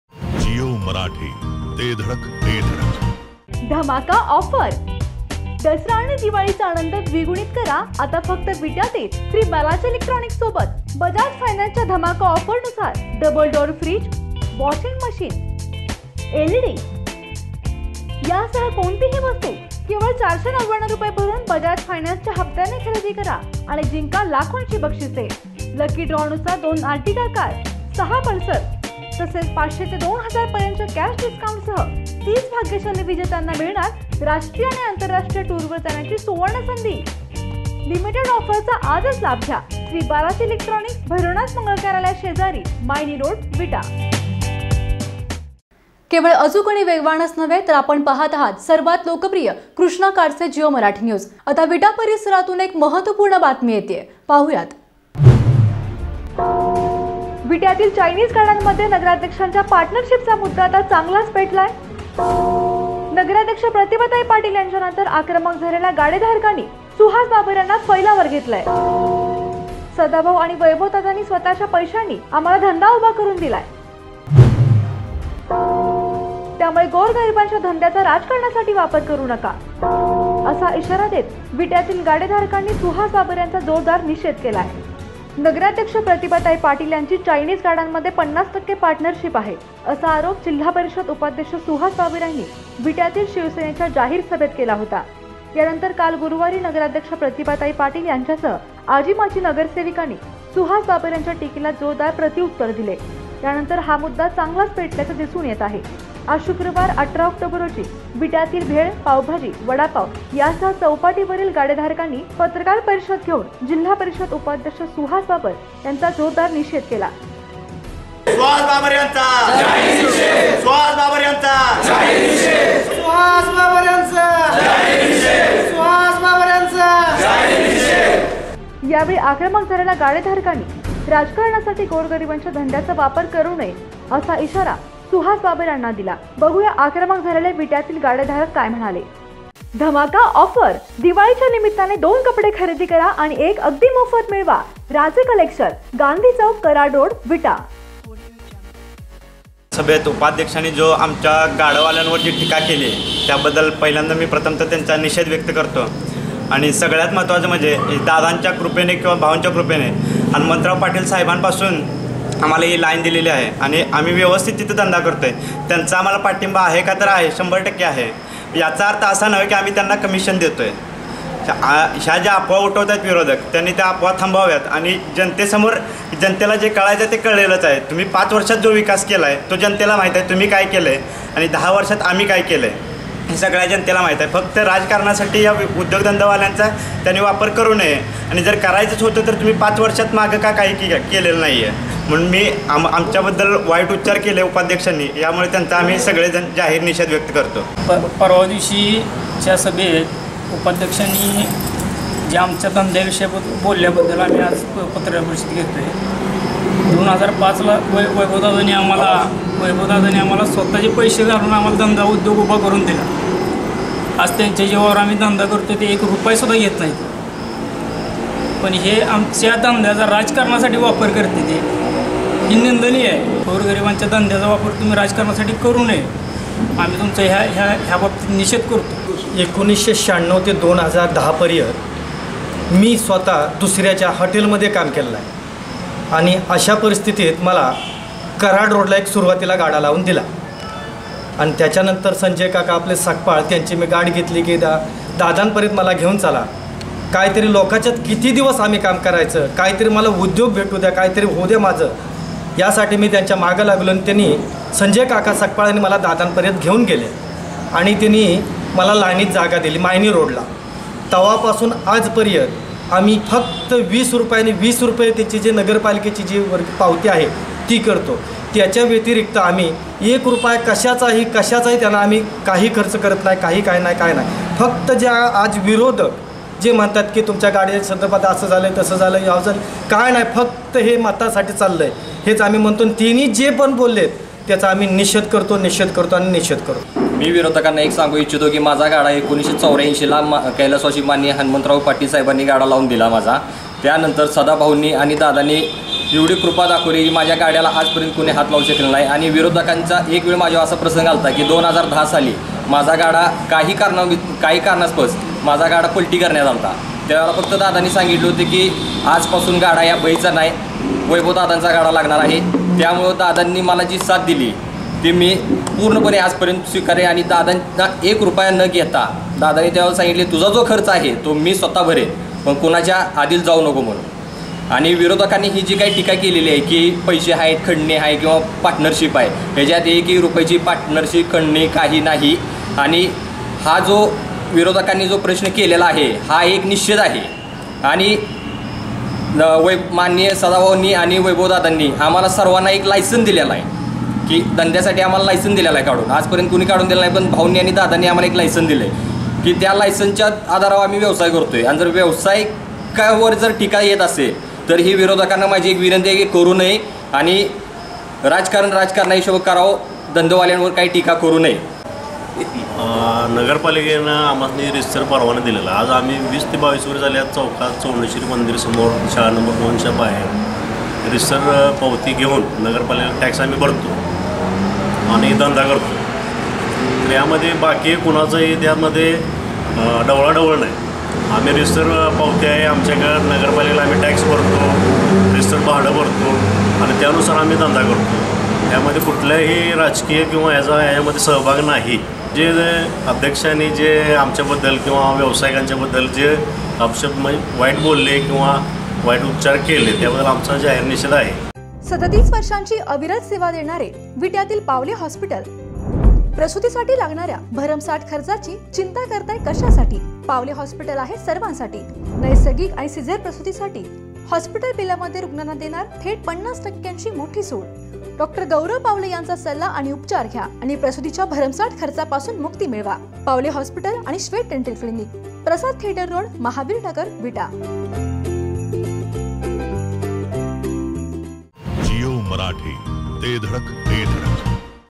■ चायनीज, भेळ व्यावसायिकांचा सुहास बाबरांवर हल्लाबोल
■ गोरगरिबांच्या धंद्याचा राजकारणासाठी वापर करू नका - सुहास बाबर यांचा विट्यात निषेध